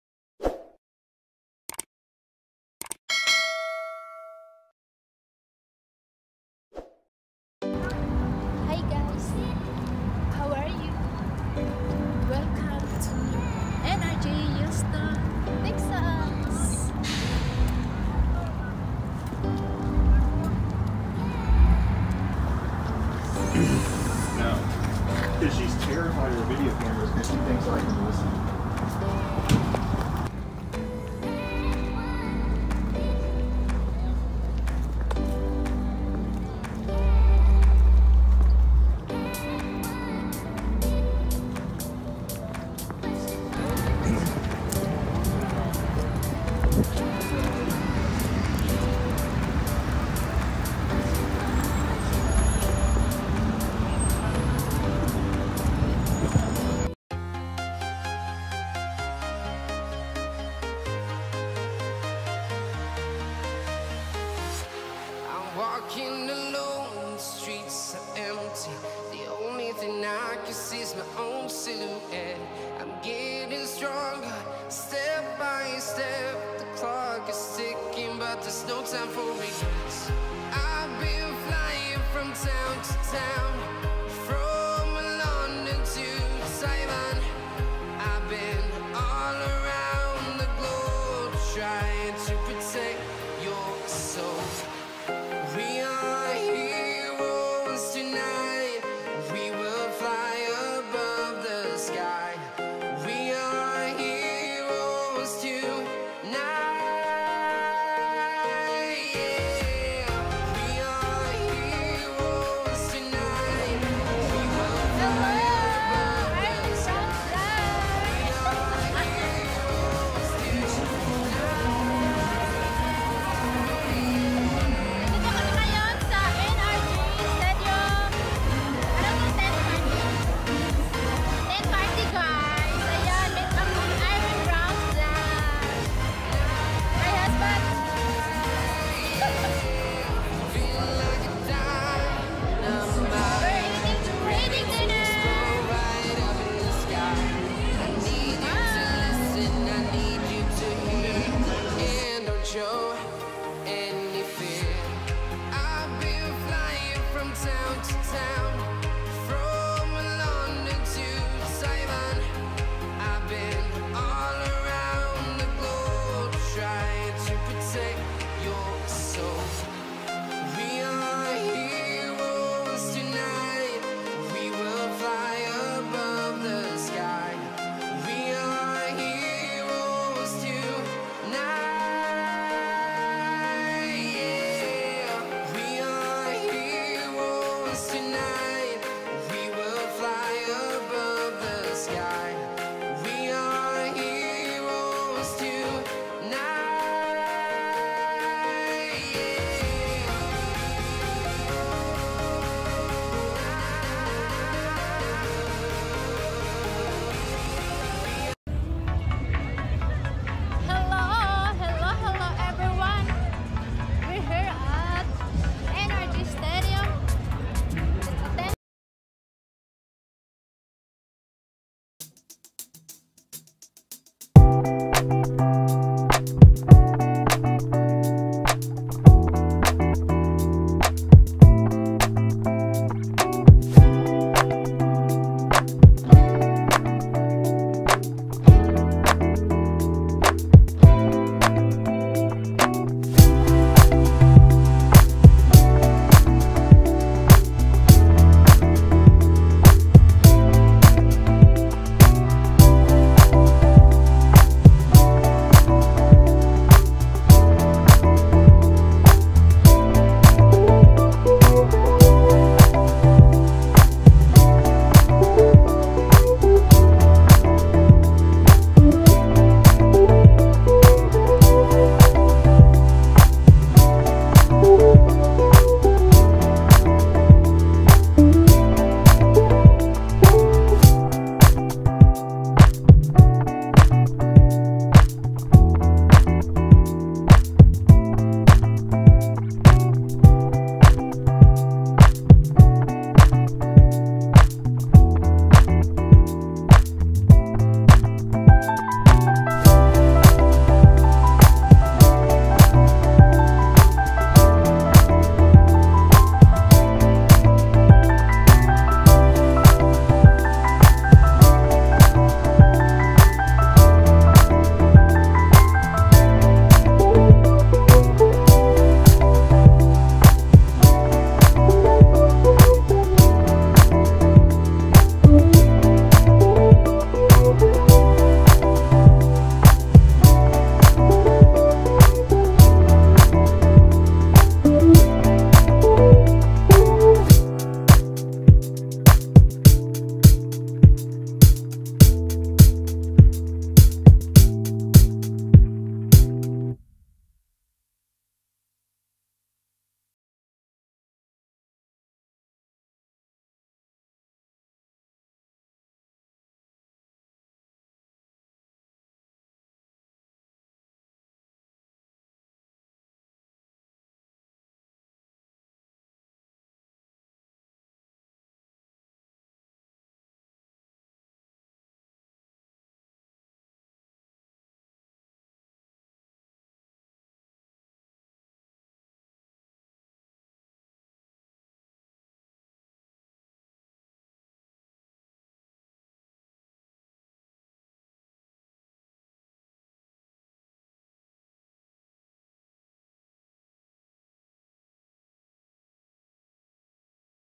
Tent party 2020 vlog 42|| Ann Wright adventures vlog
Tent party at NRG stadium in Houston Texas!